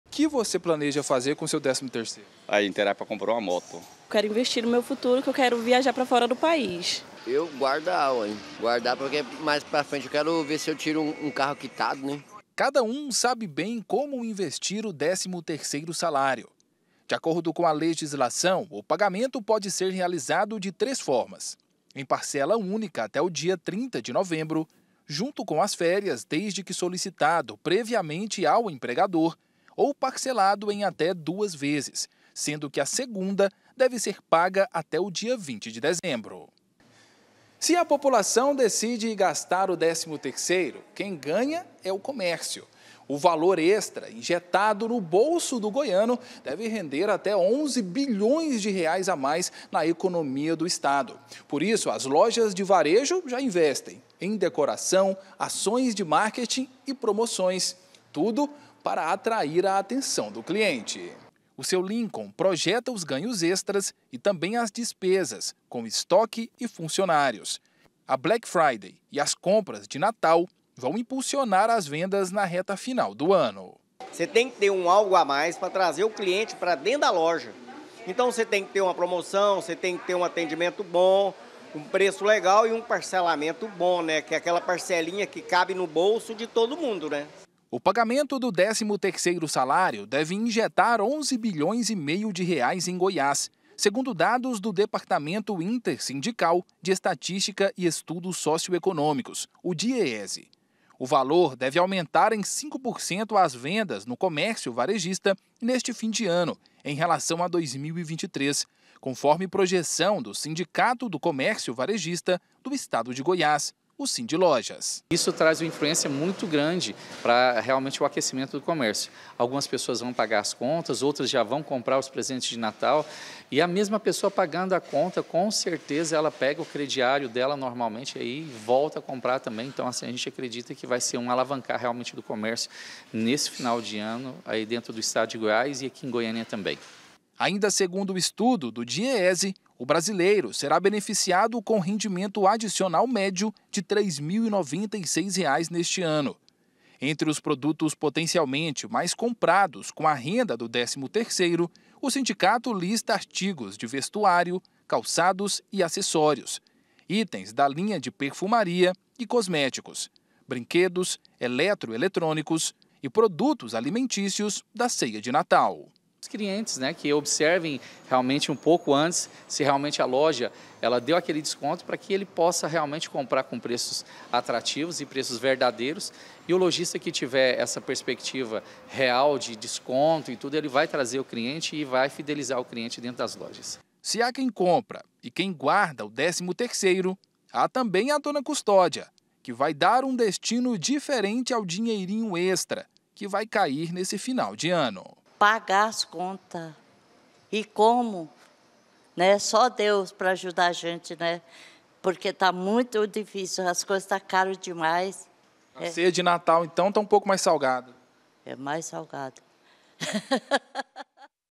CONSUMIDOR PLANEJA COMO VAI GASTAR O 13º SALÁRIO
O COMÉRCIO ESTÁ OTIMISTA COM AS VENDAS NESTE FIM DE ANO. COM AS PROMOÇÕES DA BLACK FRIDAY E A PRIMEIRA PARCELA DO DÉCIMO TERCEIRO BATENDO À PORTA, GRANDE PARTE DOS CONSUMIDORES PLANEJA IR ÀS COMPRAS. A PREVISÃO DO SINDILOJAS É DE AUMENTO NAS VENDAS ATÉ DEZEMBRO.